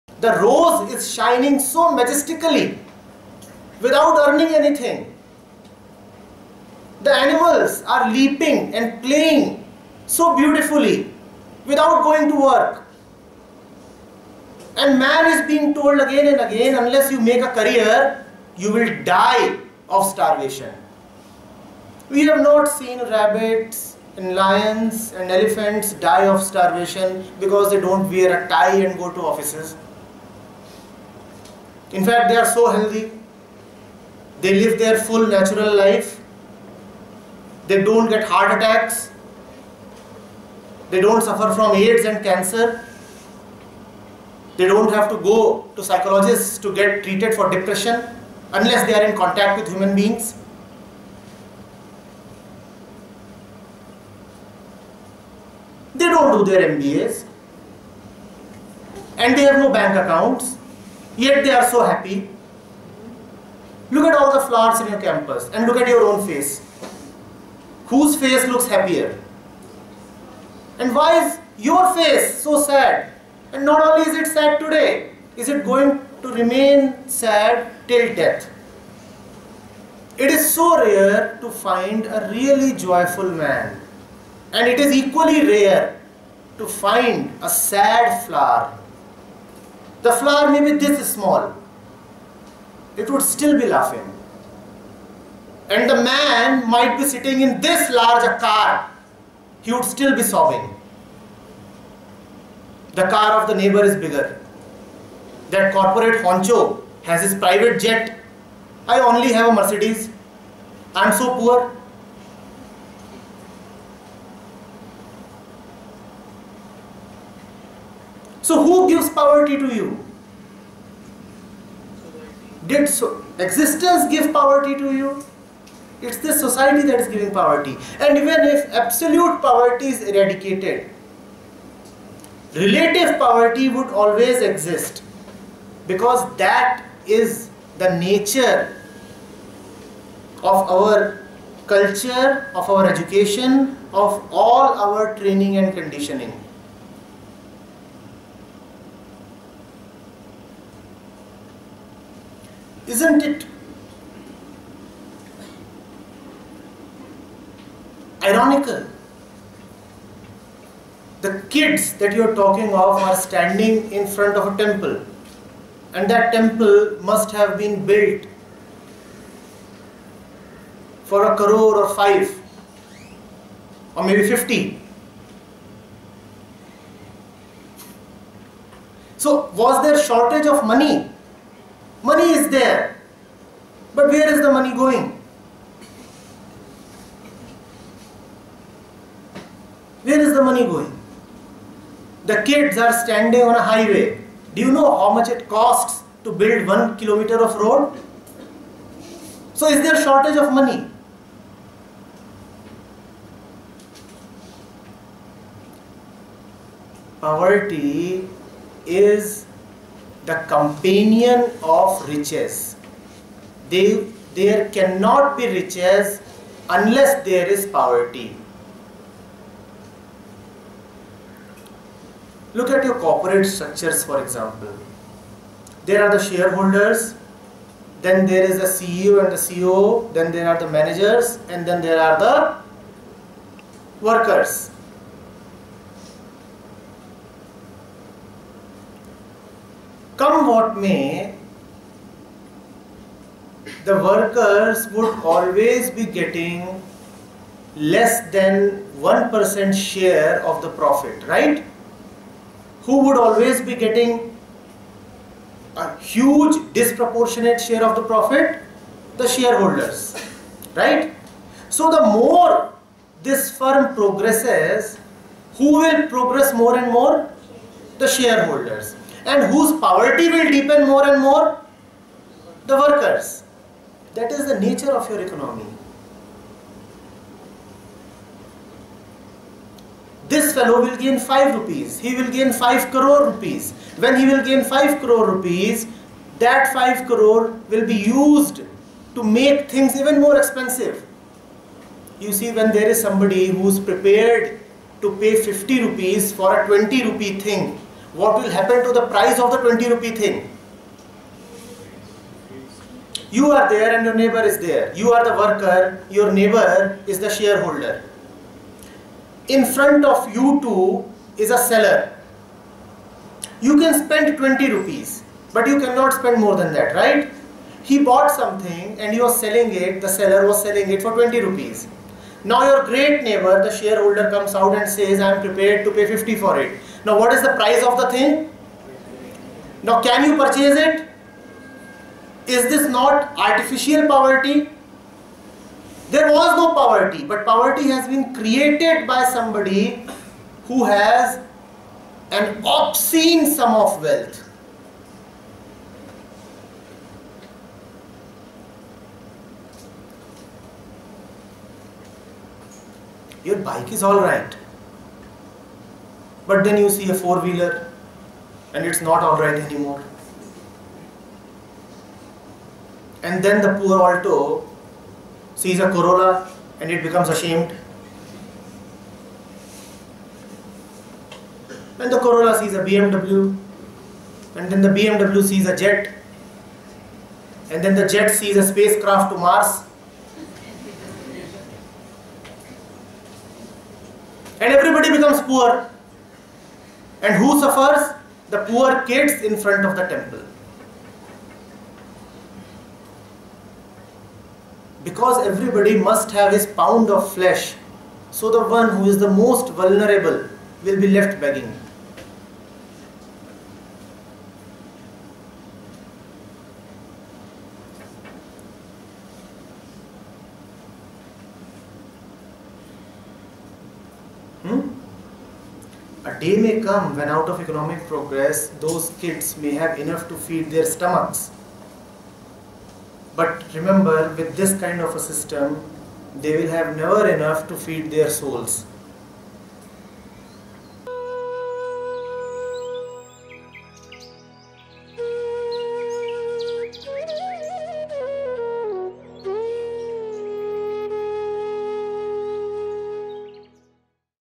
How we create poverty || AP Neem Candies
Context:
~ Why is poverty existent?
~ How to eradicate poverty?
~ What is richness?